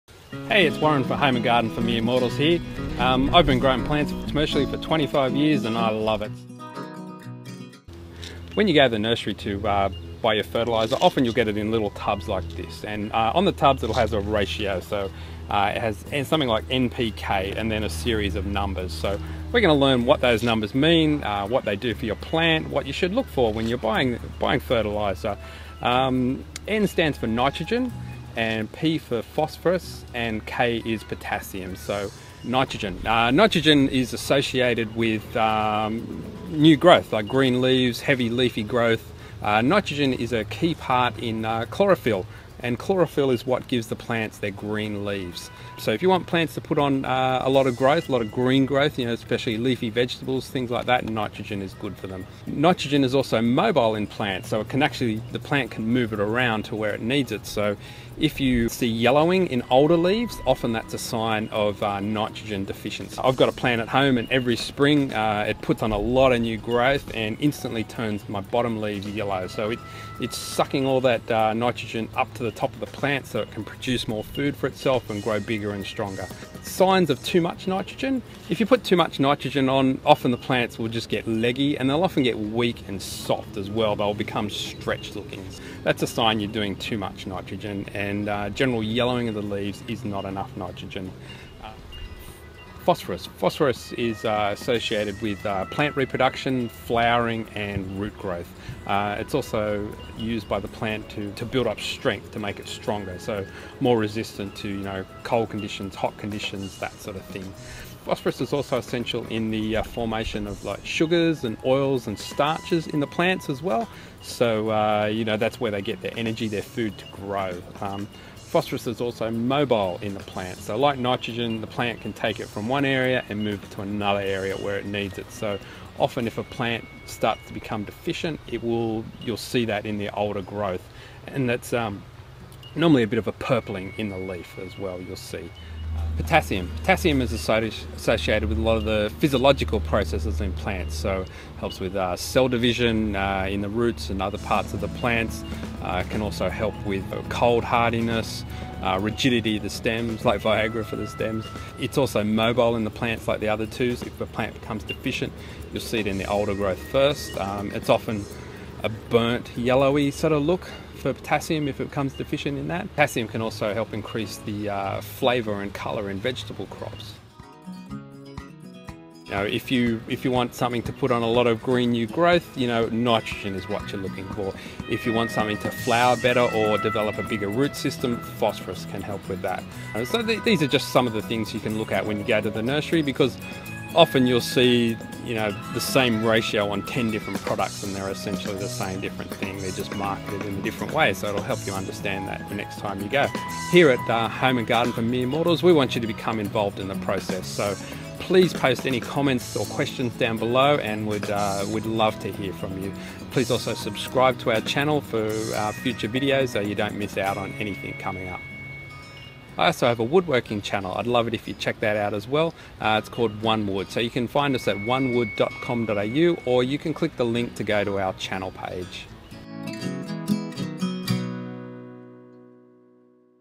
What are those N•P•K numbers on plant food and fertilizer?
Please welcome Warren Downes to HGMM! If you've done any gardening, you've probably seen an "NPK" rating listed on boxes of plant fertilizer. What should you be looking for?

Warren has been commercially growing plants for over 26 years in his wholesale nursery and in that time has developed a wealth of knowledge of plant growth and the horticulture industry.

For Mere Mortals is a trademark of